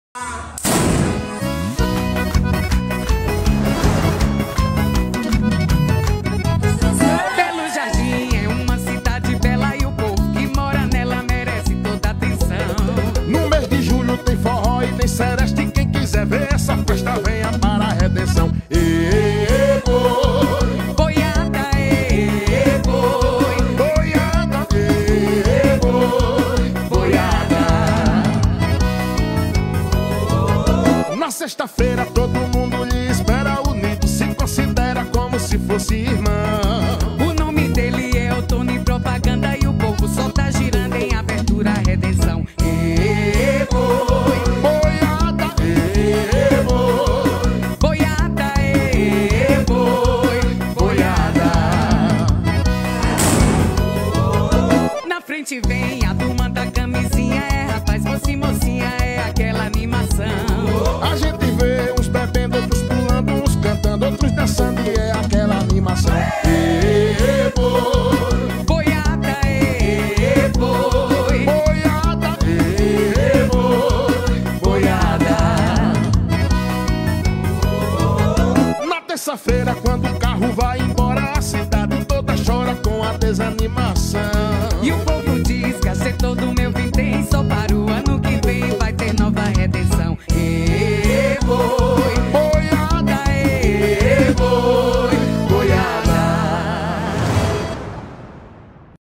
Belo Jardim - Terra das Marocas
Belo Jardim é um município do Agreste de Pernambuco, que possui a tradicional Festa da Redenção (Festa das Marocas).